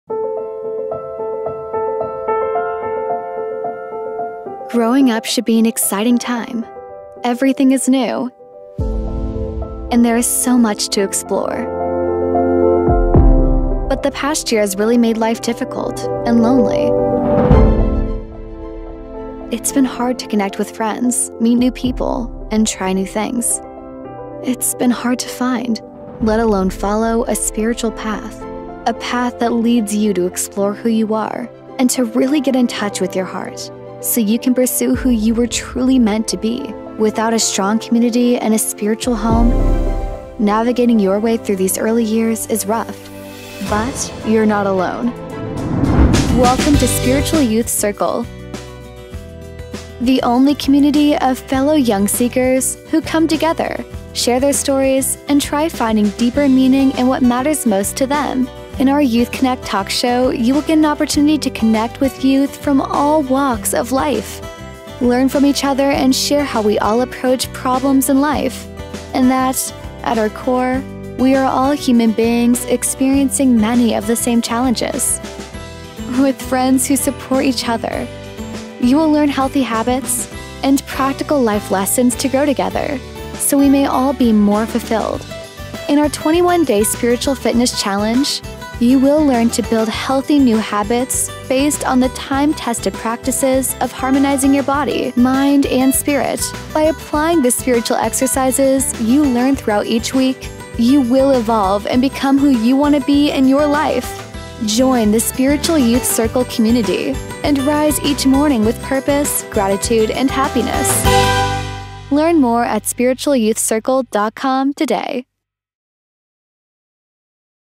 Spiritual Youth Circle Overview
Inspire Hope, Heart And Happiness
SPIRITUAL YOUTH CIRCLE

Spiritual Youth Circle is an online community for youth to come together and share from the heart, build genuine connections, develop habits to lead a purposeful life, and most importantly have fun.